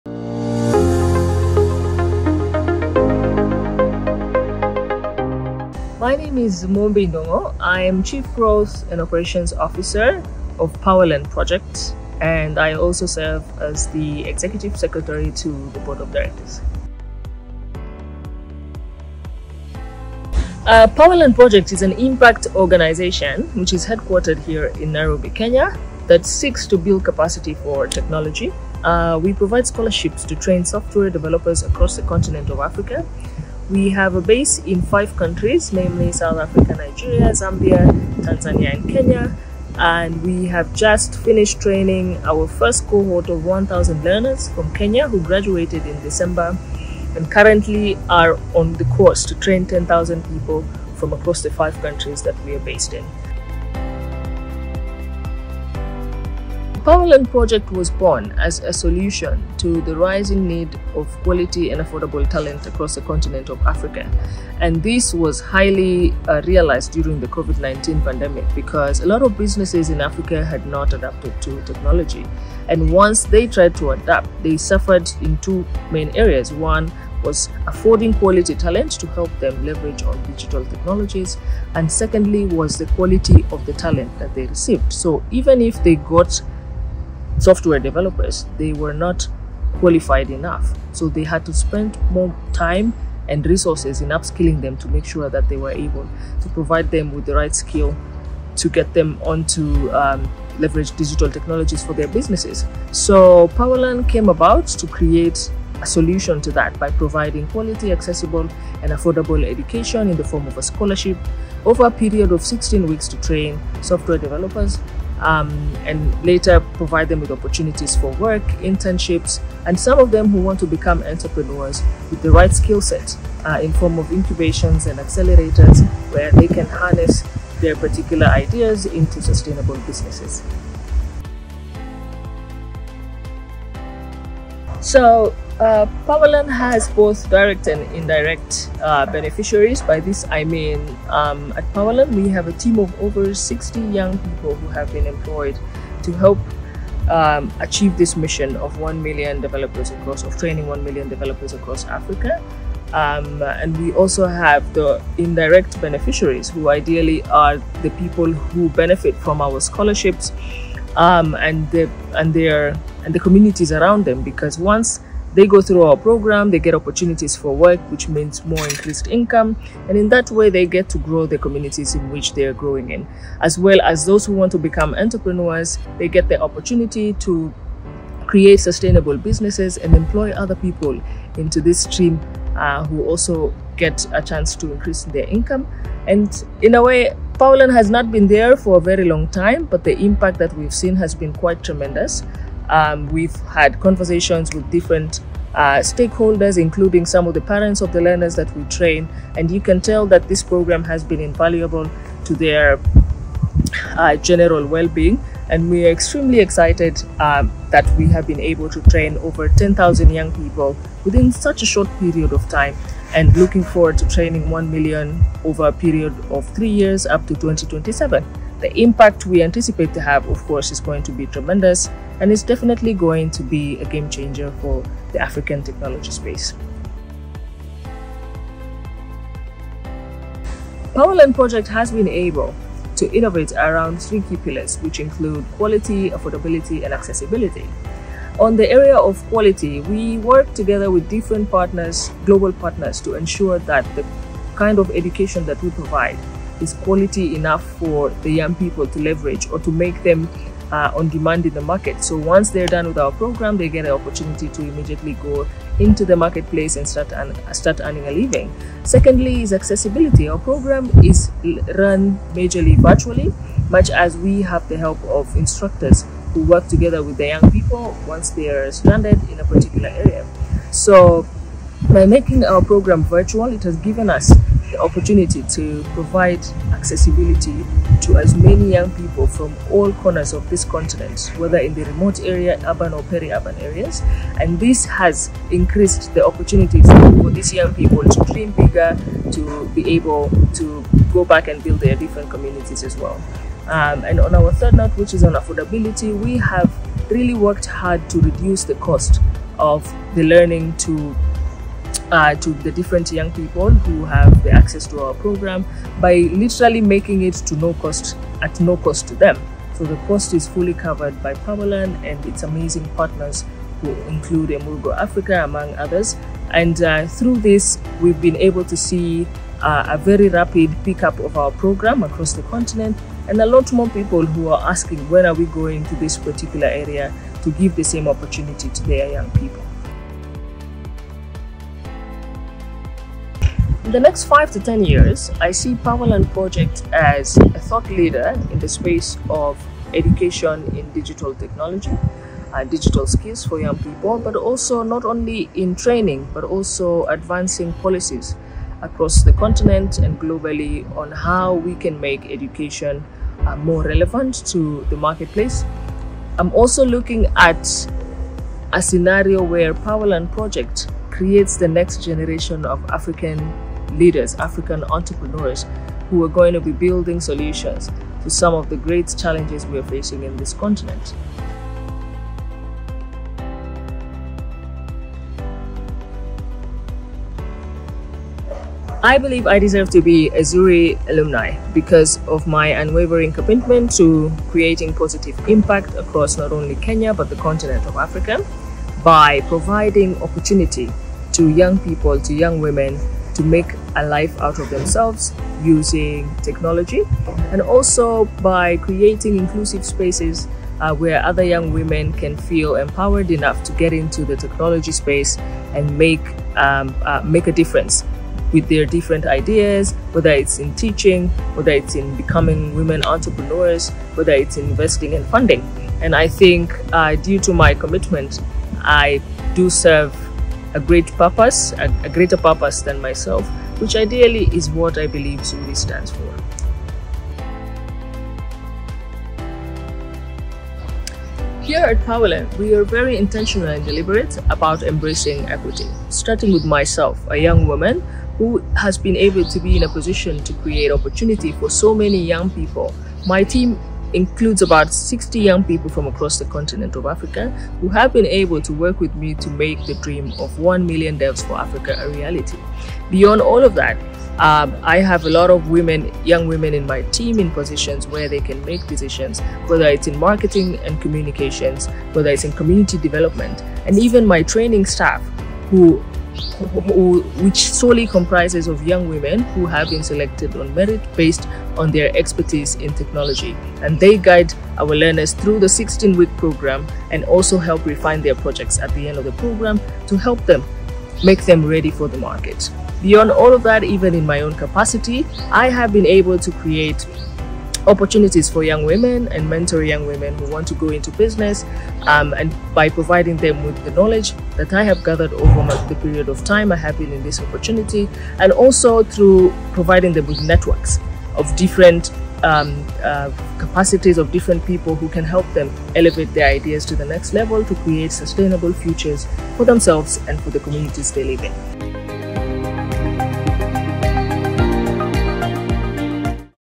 Mumbi Ndung'u-STEM Honoree
Mumbi Ndung'u ZuriAwards 2023 STEM Honoree is a social innovation expert and leader having launched several enterprises since university, recent innovation is a tech learning platform dubbed: Power Learn Project (PLP) is a Pan-African social impact organization whose goal is to drive transformative change for youth in Africa by powering them with relevant Tech skills through the provision of quality and decentralized Tech training.